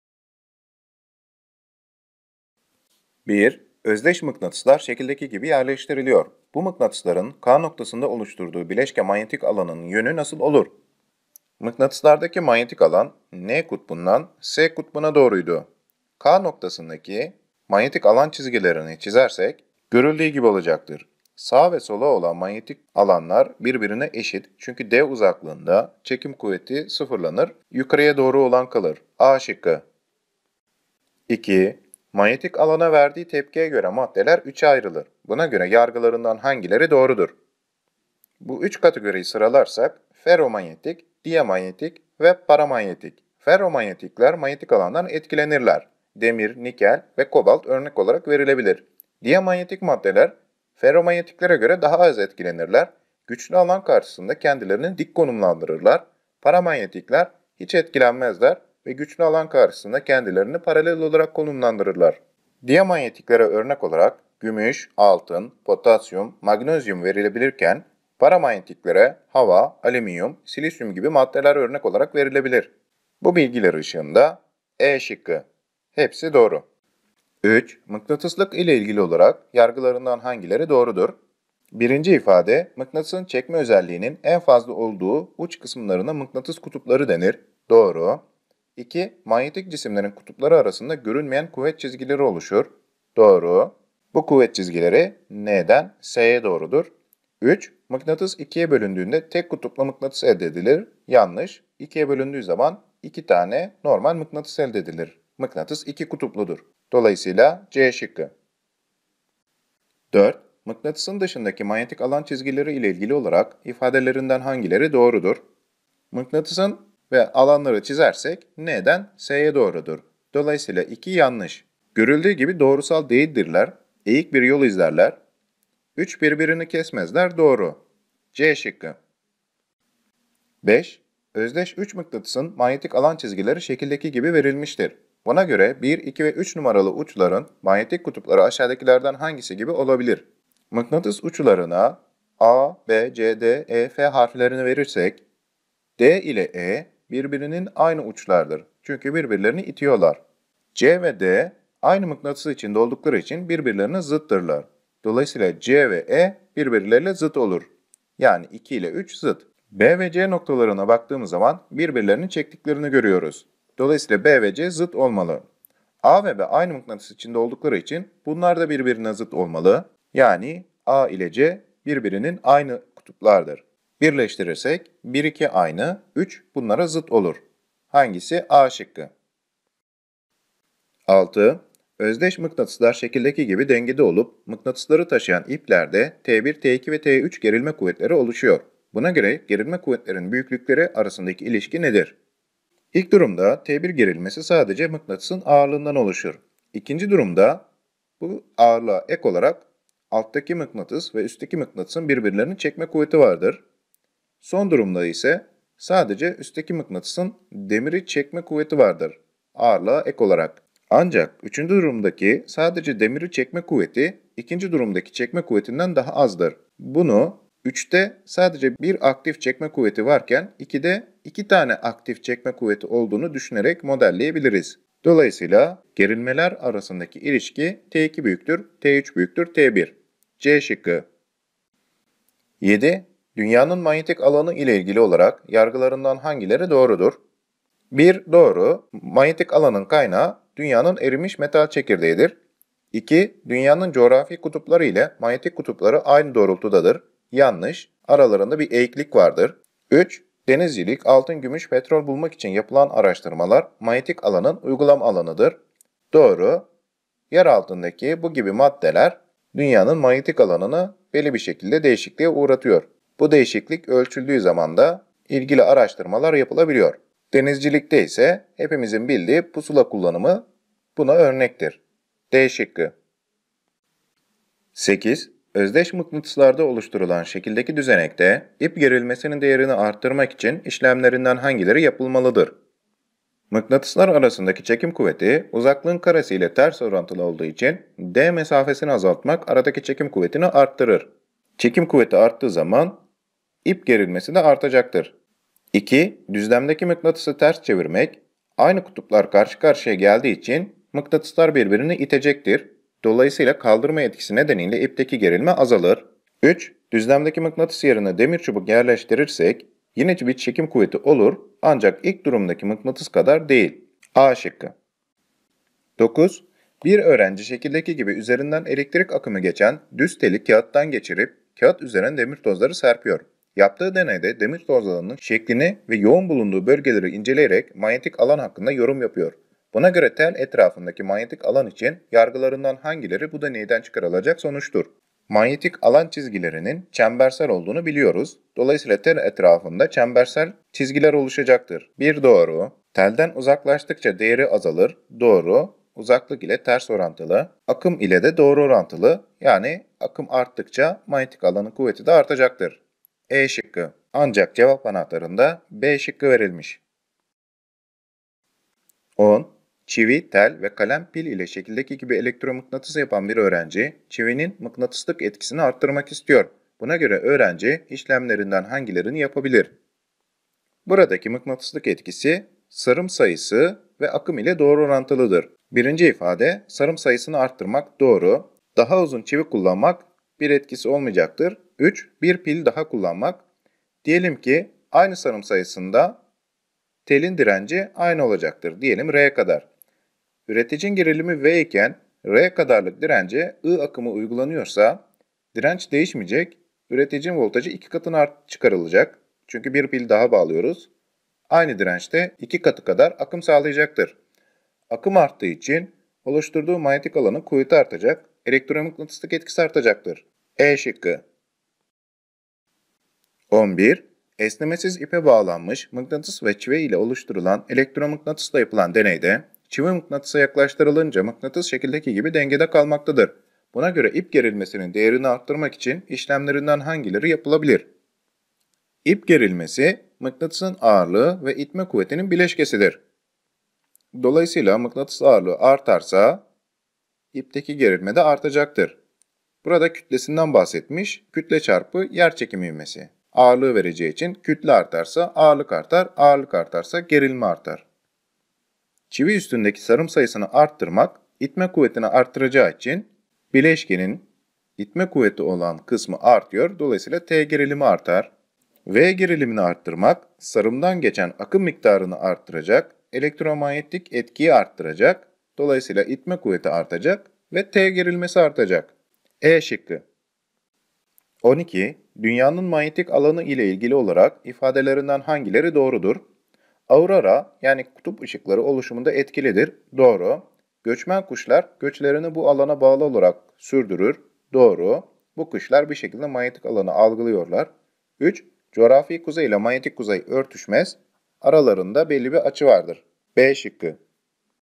10. Sınıf Fizik Test 10 - Elektrik ve Manyetizma 5
MEB 10. Sınıf fizik kazanım kavrama test 10 çözümü, elektrik ve manyetizma 5, mıknatıslar, manyetik alan, elektrik ve manyetik alan, elektromıknatıs, dünyamızın kutupları